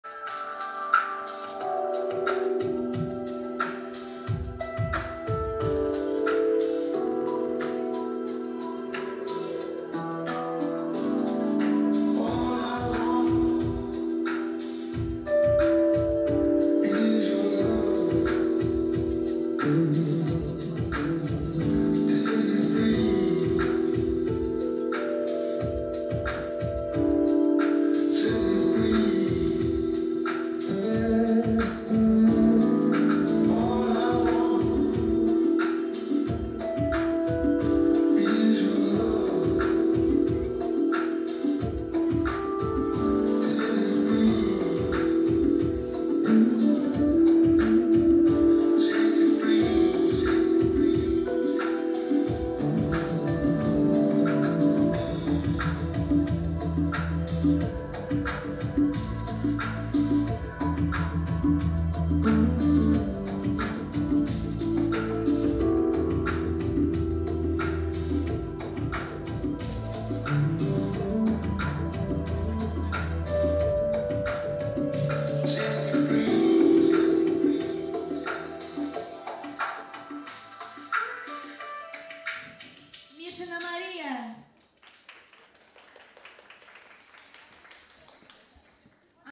Селезнева Яна 2 место, стрип соло, 6 ой Dance Star Festival Любители 27 апреля 2014
6-ой Dance Star Festival - Любители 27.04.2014.